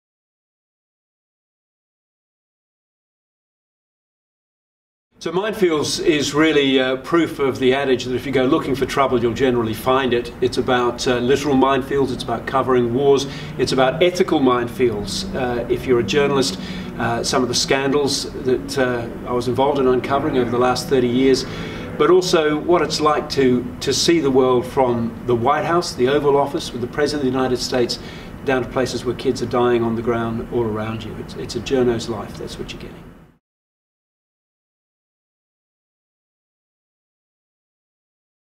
Story of Minefields by Hugh Riminton
Every now and then it’s not so bad to go looking for trouble. Just ask Hugh.

MINEFIELDS is Hugh Riminton's fascinating memoir of over forty years on the frontline of the news game.